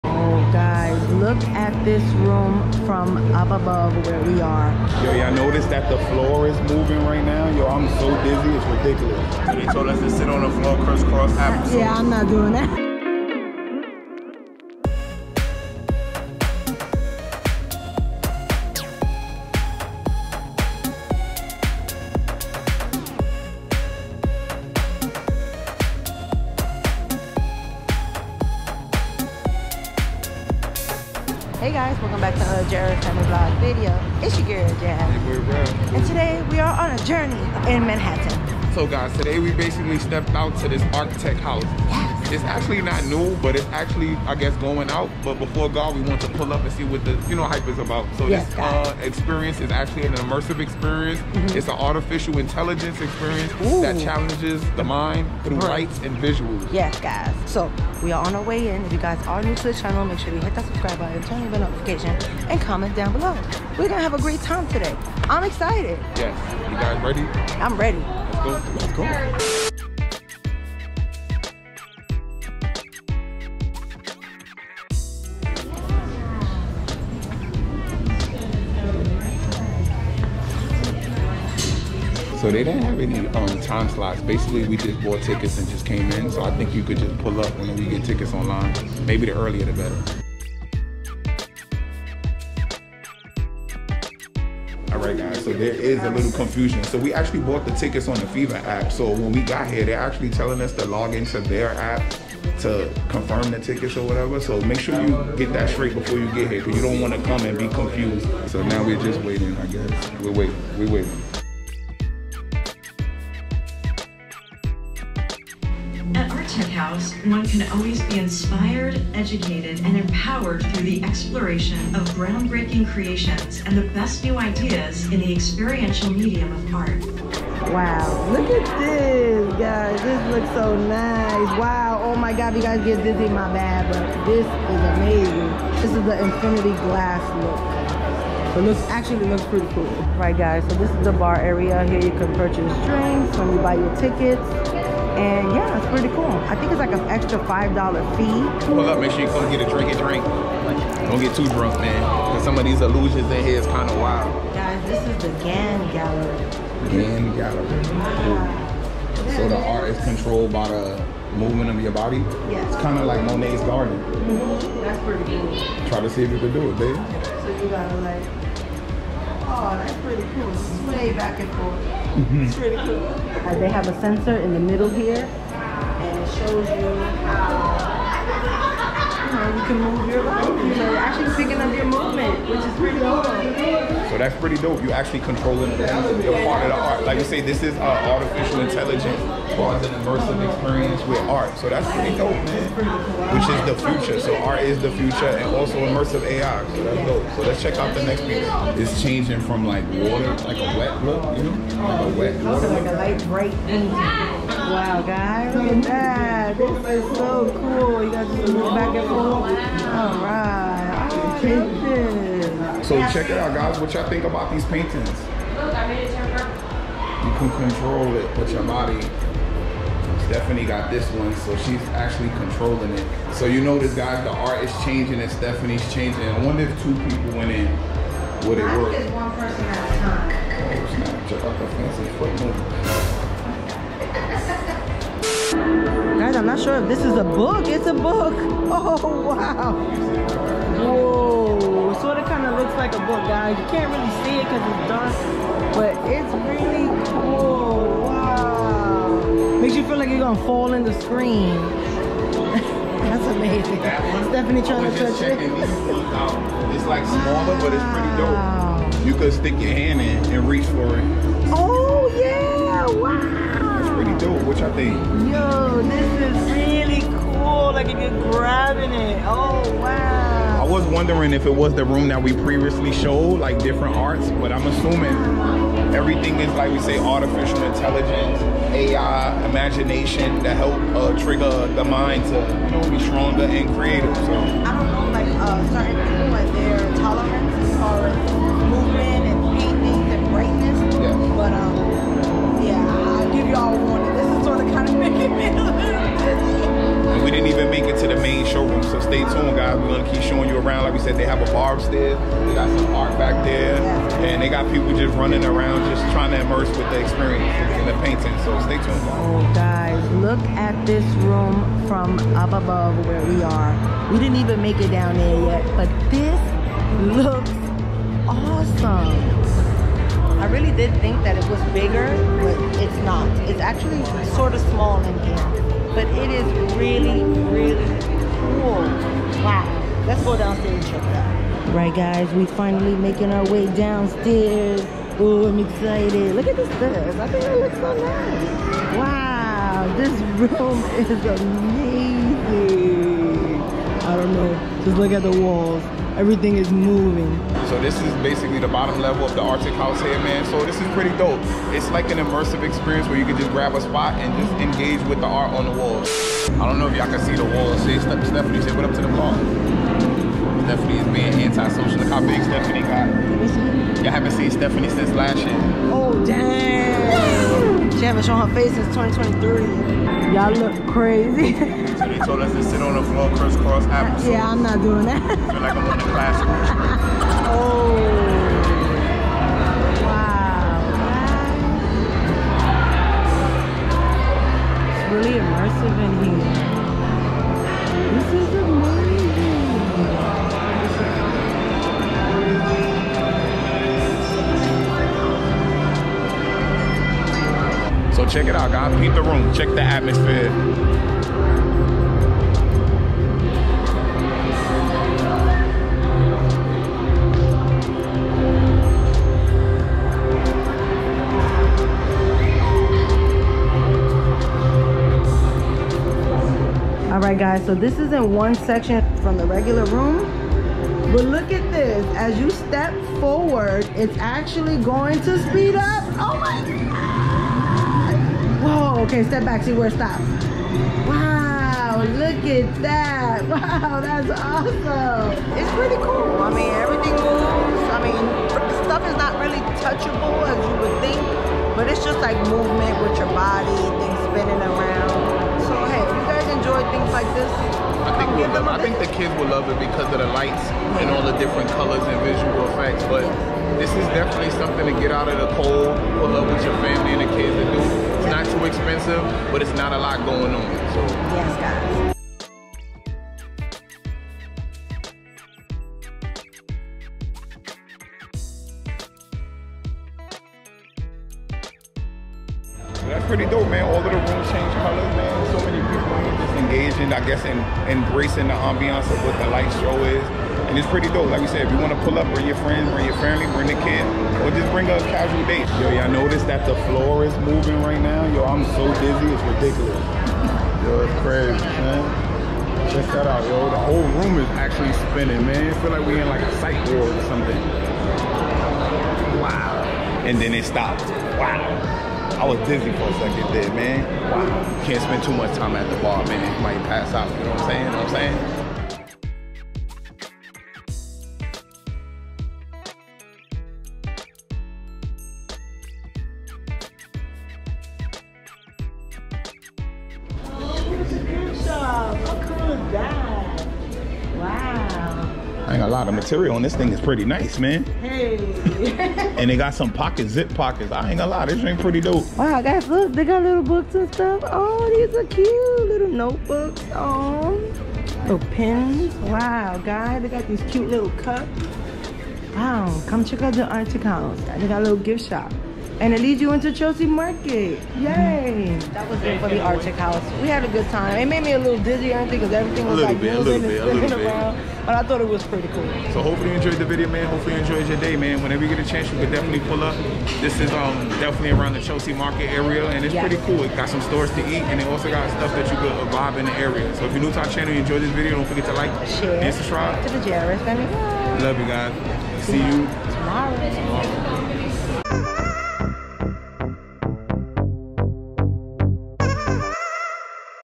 Artechouse - AI·MAGINATION | NYC
TODAY WE ARE AT ARTECHOUSE EXPLORING THE WORLD OF AI MAGINATION. ARTECHOUSE IS THE NATION'S FIRST INNOVATIVE ART ORGANIZATION DEDICATED TO THE INTERSECTION OF ART, SCIENCE AND TECHNOLOGY. COME WITH US AND LETS HAVE SOME FUN.